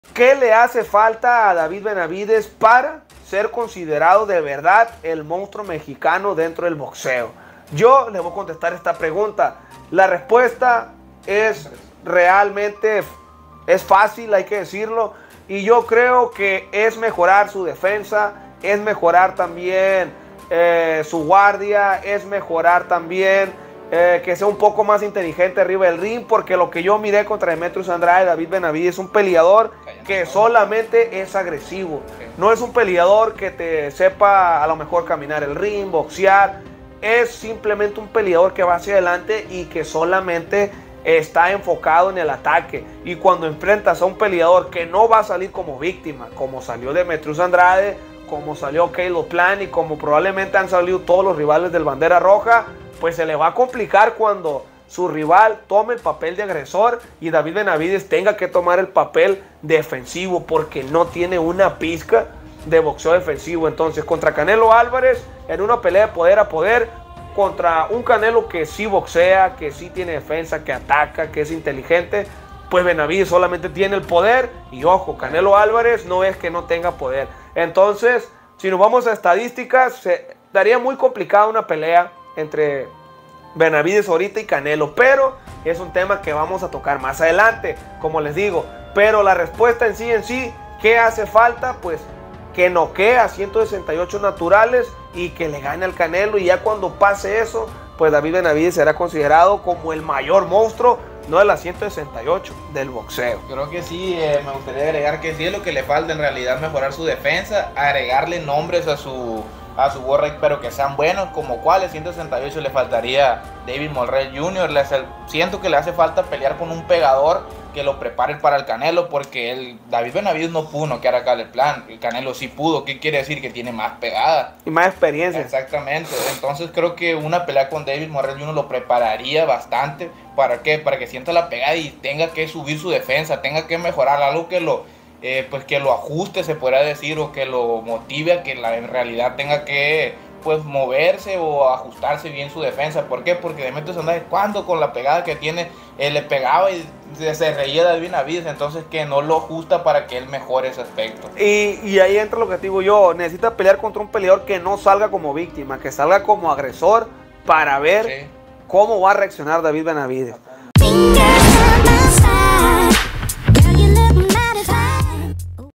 ¿Que le HACE FALTA a David Benavidez para CONVERTIRSE en el MONSTRUO que todos creen?
Te EXPLICAMOS que NECESITA MEJORAR David Benavidez para SER UN MONSTRUO‼️